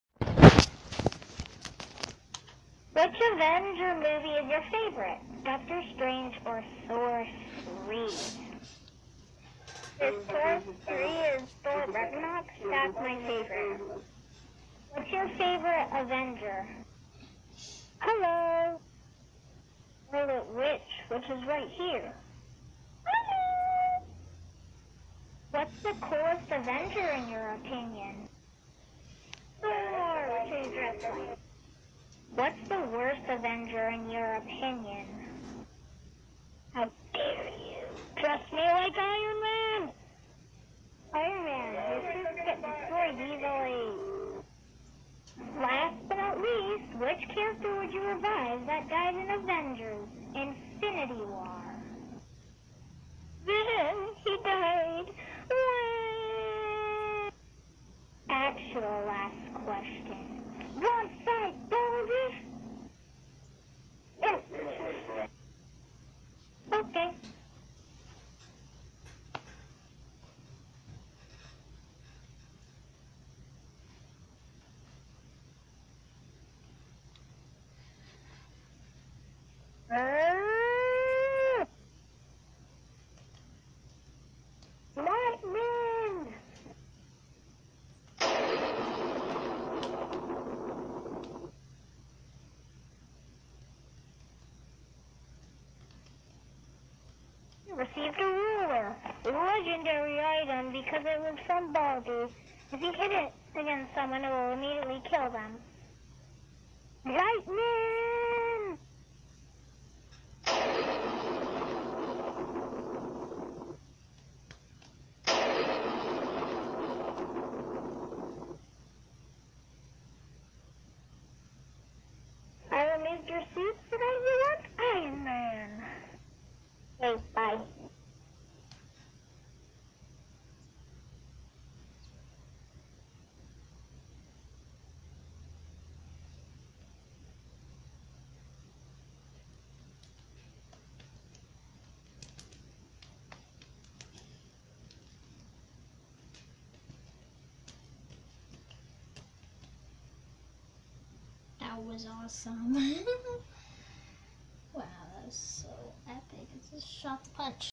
Anitales: "Avenger interview destructivestudios3" - lognab
''Which avenger movie is your favorite? Doctor Strange or Thor 3', lognab said'. 'Avenger interview destructivestudios3' created by lognab using the Anitales app.